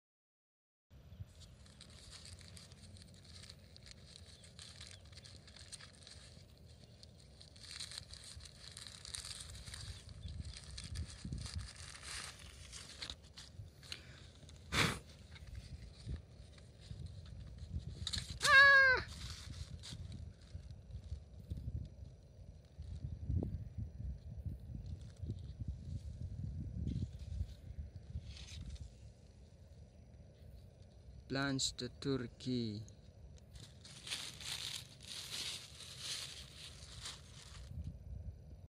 Blanche de turquie - Turkey fig variety - Buah Tin ara
Fig variety from turkey,
But the source by Harvey C
Big sweet taste, green and yellow colors gradations, very vigorous, and rare fig

By. Wonderfigs Garden